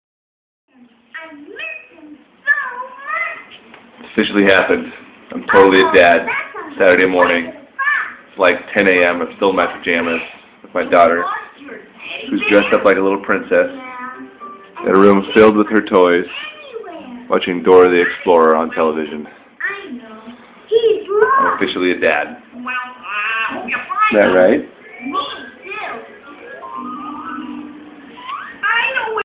Officially a Dad
Adele and I watching Saturday morning cartoons.